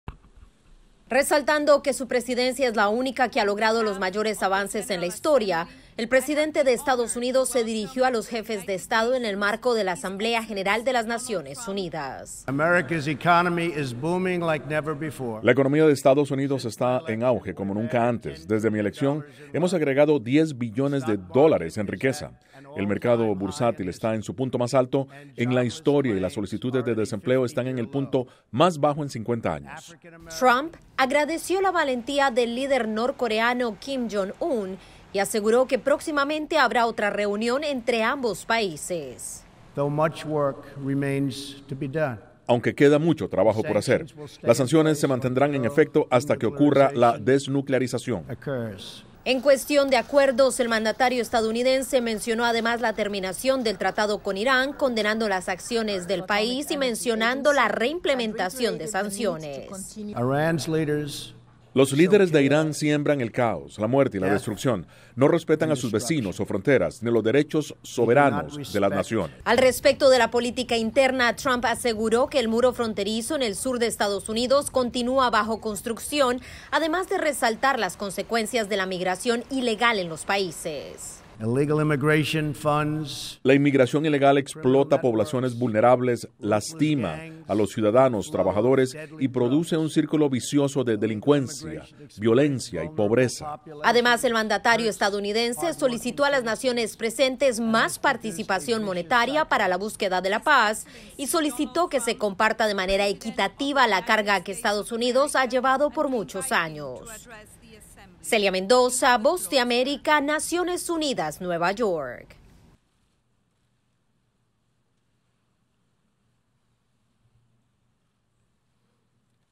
El mensaje de Trump en la ONU
Enumerando sus éxitos y hacienda una invitación a la cooperación entre las naciones, el presidente de Estados Unidos Donald Trump se dirigió por segunda vez durante la Asamblea General de las Naciones Unidas.
Publicado originalmente